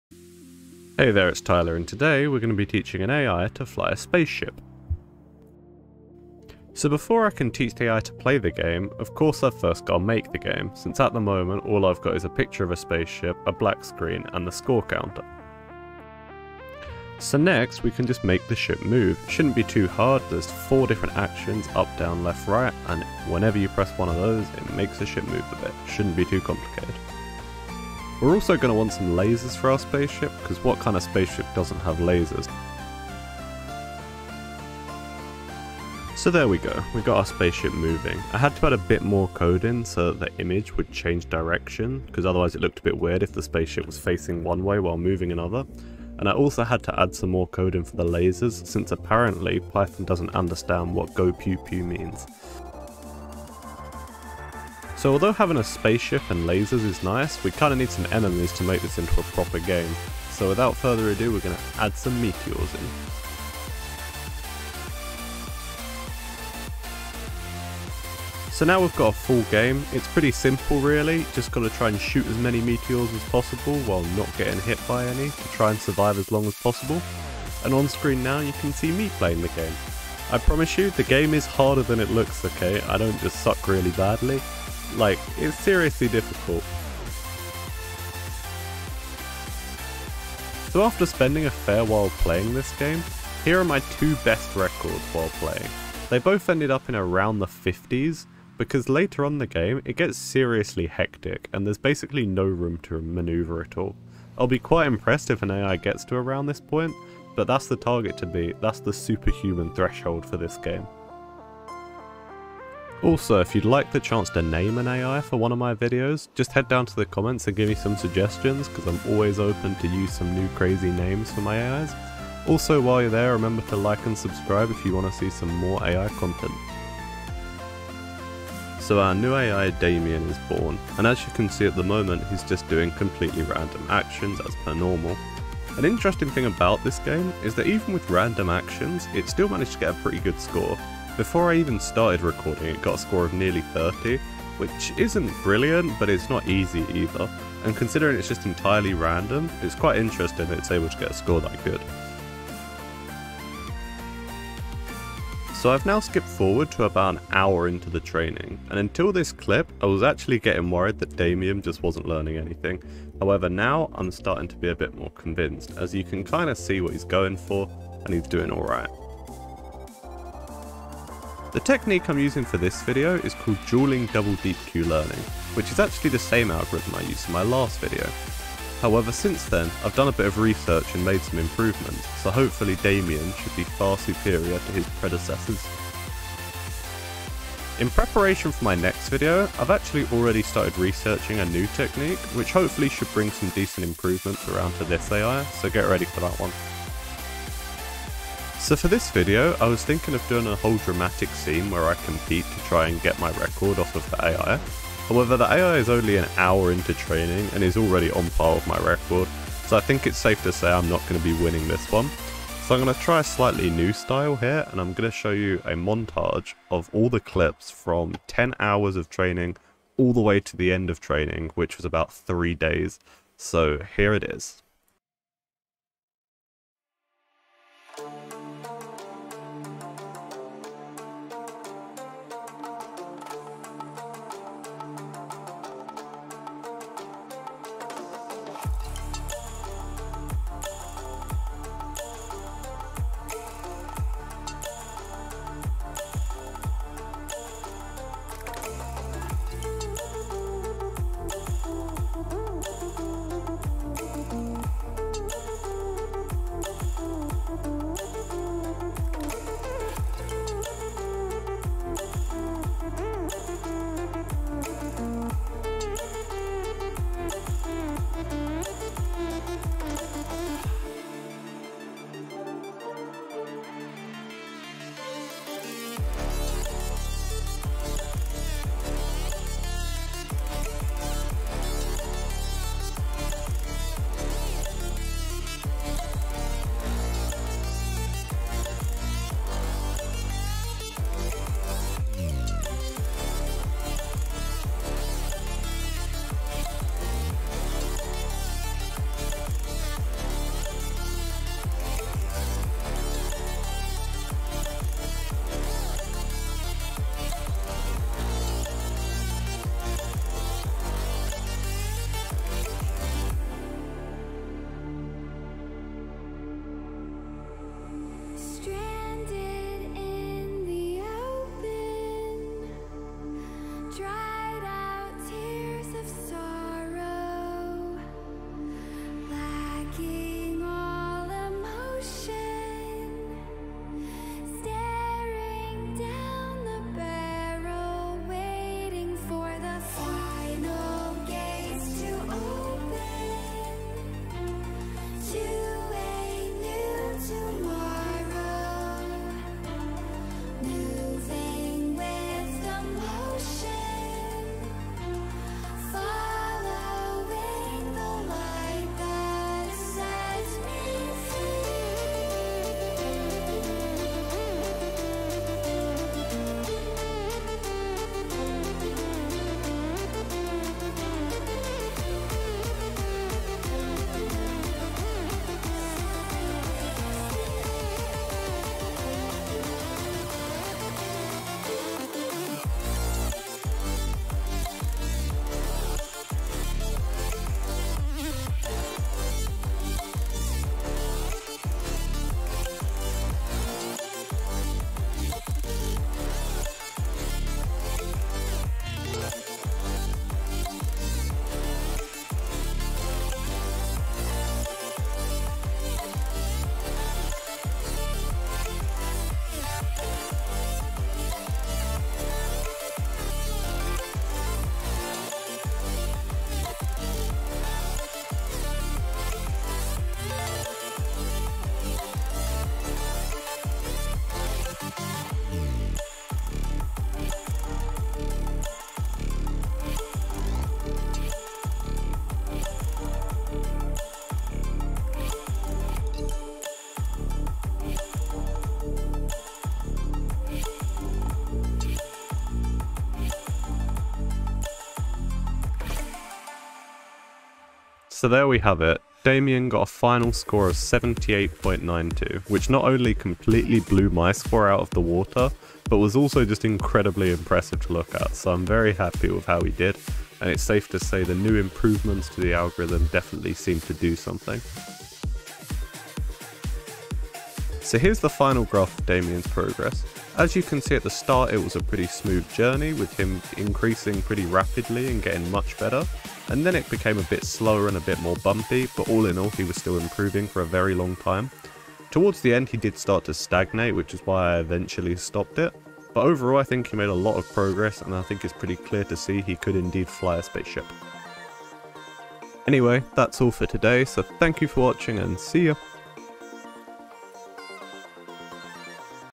AI Learns to FLY a SPACESHIP
An AI named Damien learns to fly and pilot and spaceship with lasers via Deep Reinforcement Learning. After many hours of Machine Learning, he attempts to become Superhuman.

Artwork by John B. Marine.